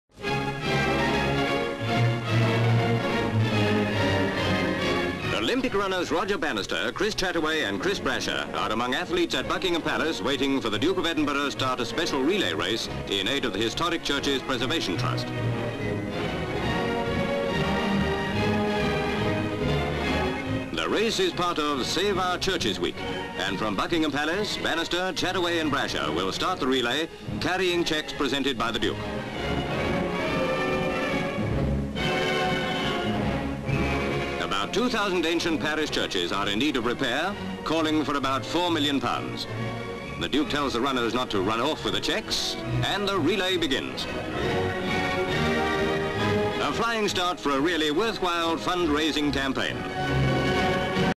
Duke Starts Charity Race (1955)
Buckingham Palace, London.

SV. Runners lined up in forecourt of Buckingham Palace waiting for Duke of Edinburgh to start a special relay race in aid of Historic Churches Preservation Trust.  Roger Bannister, Chris Chataway, Chris Brasher, etc. SV. Duke accompanied by Ivor Thomas walking towards sportsmen. CU. Bannister and Chataway. SCV. Prince Philip shaking hands with Bannister, Chataway and Brasher. SCU. Duke takes cheque from Mr Thomas and hands it to Bannister, then gives cheques to the other two runners. SV. Bannister, Chataway and Brasher start to run, pan with them until they reach Palace gates.

(Lav.) (Orig.D.)
 FILM ID:519.25